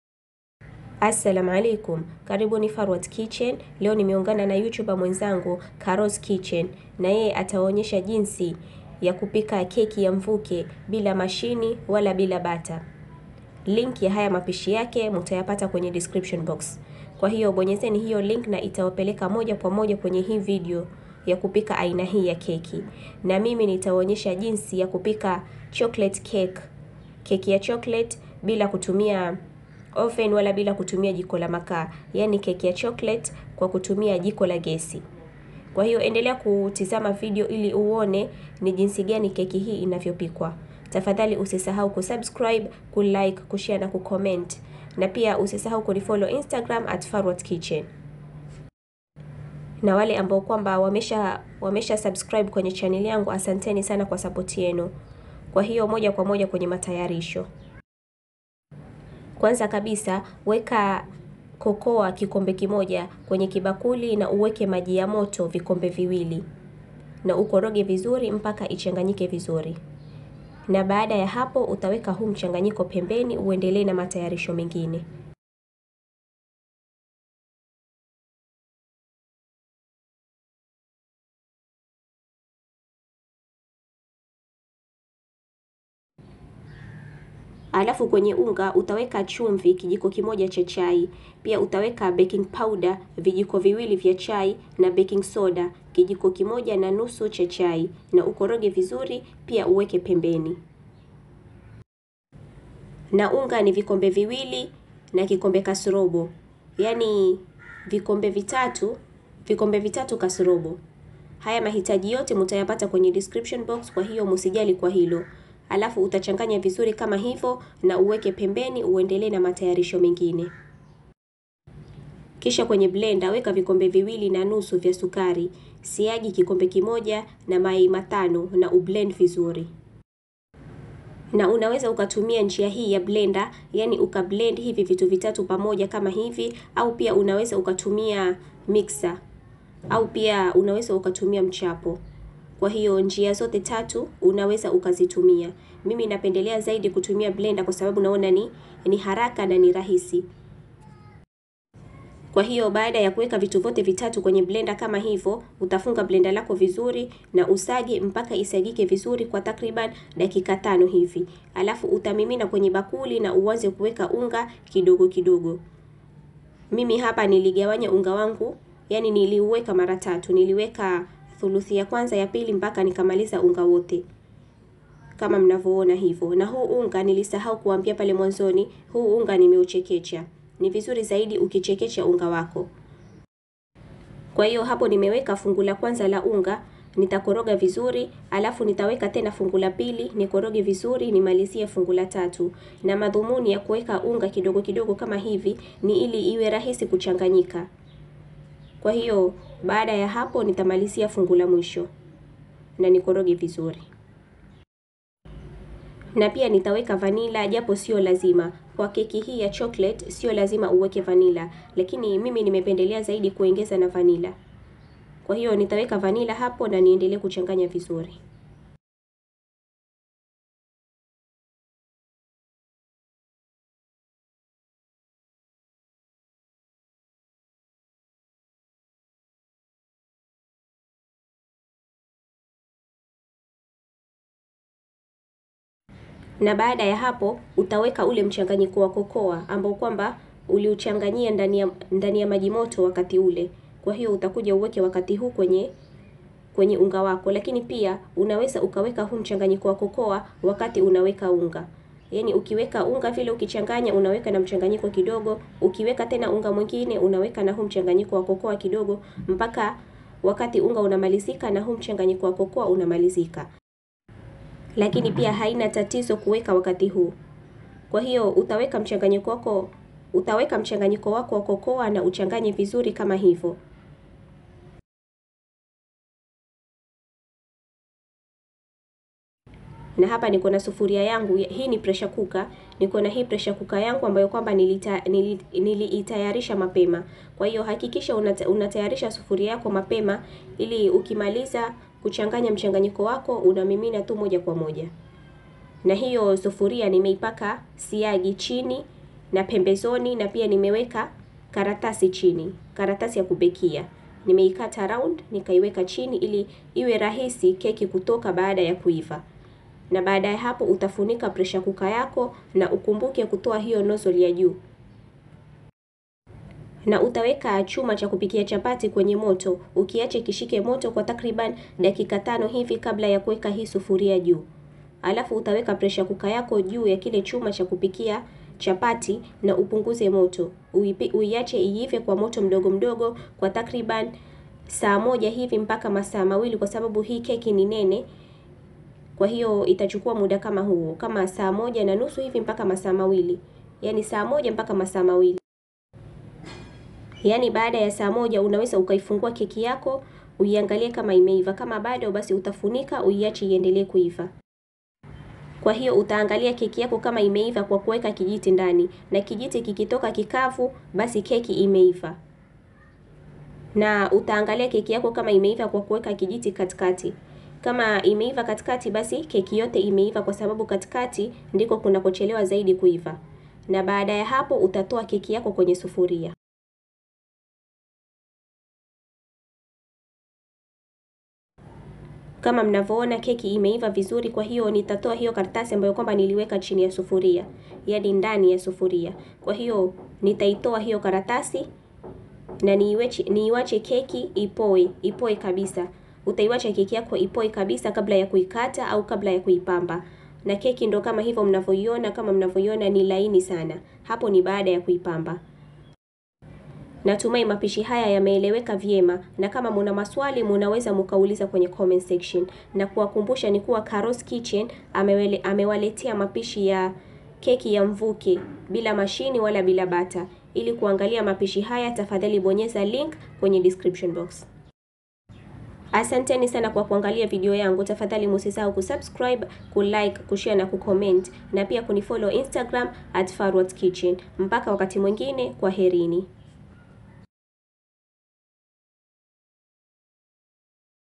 JINSI YA KUPIKA KEKI YA CHOCOLATE INAYOCHAMBUKA KWA JIKO LA GESI(COLLABORATION WITH CARO'S KITCHEN)
MAHITAJI YA KEKI YA CHOCOLATE
▪️Unga wa ngano 2 & 3/4 cups
▪️5 eggs
▪️Margarine 1 cup
▪️sugar 2 & 1/2 cups
▪️Cocoa 1 cup
▪️Maji ya moto 2 cups
▪️Baking powder 2tsp
▪️Baking soda 1 & 1/2 tsp
▪️Salt 1tsp